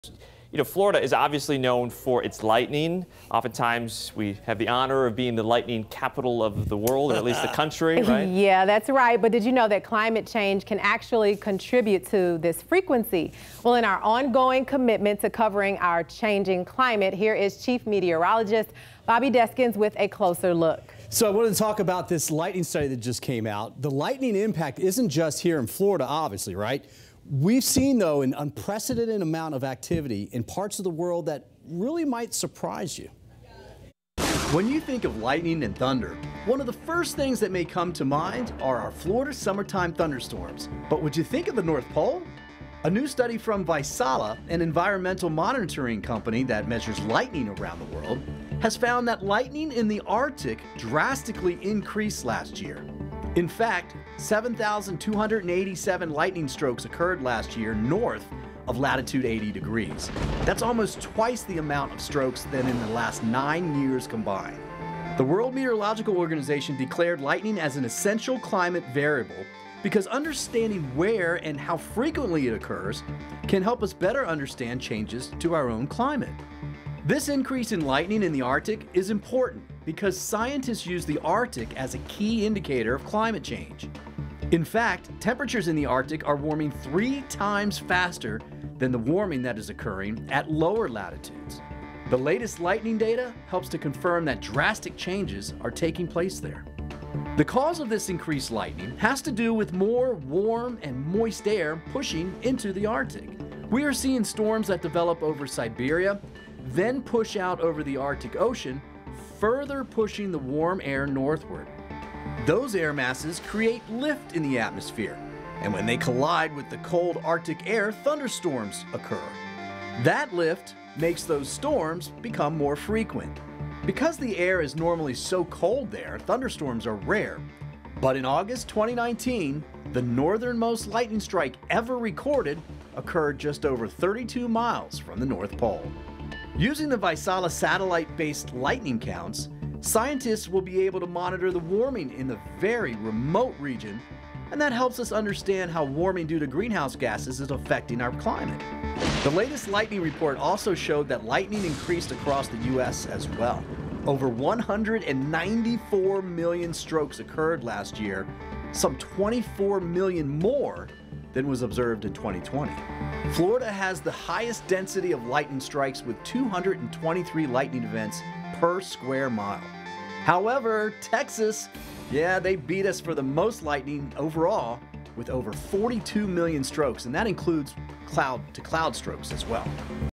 Lightning in the Arctic increased drastically last year
The increase in lightning in the arctic is a sign of the warming that is occurring in that region